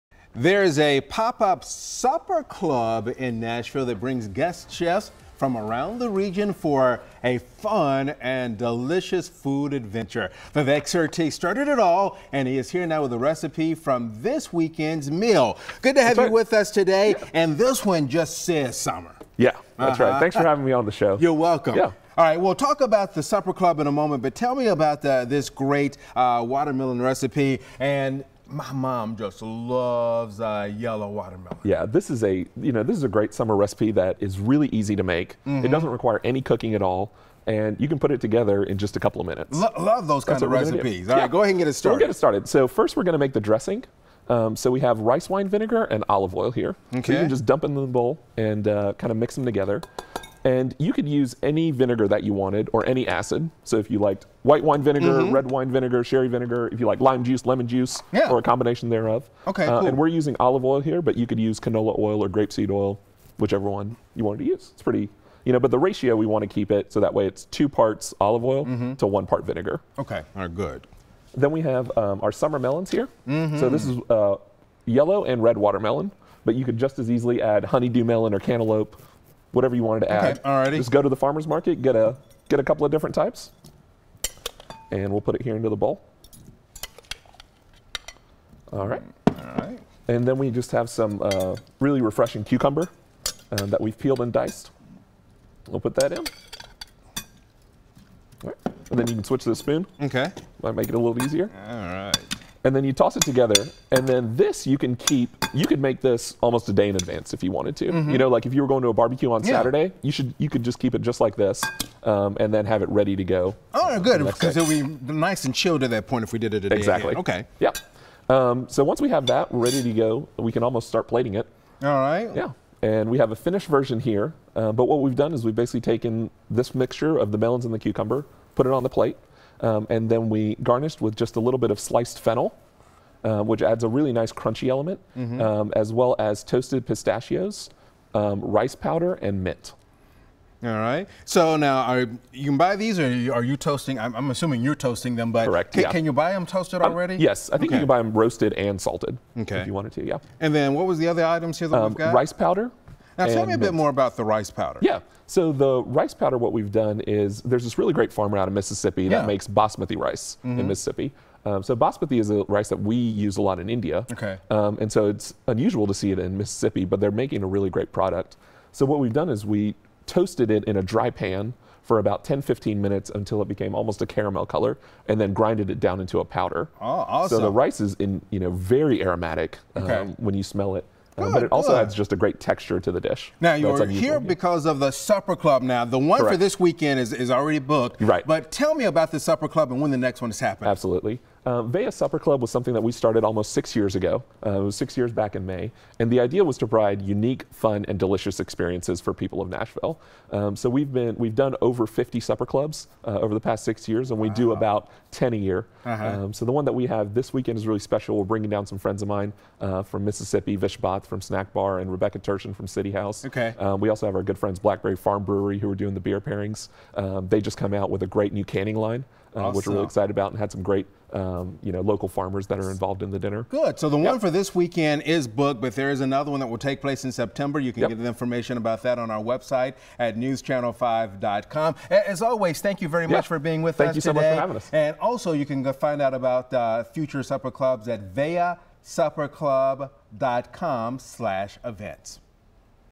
VEA Supper Club Preview Recipe: Summer Melon & Cucumber Salad
Vivek Surti with VEA Supper Club Preview Recipe: Summer Melon & Cucumber Salad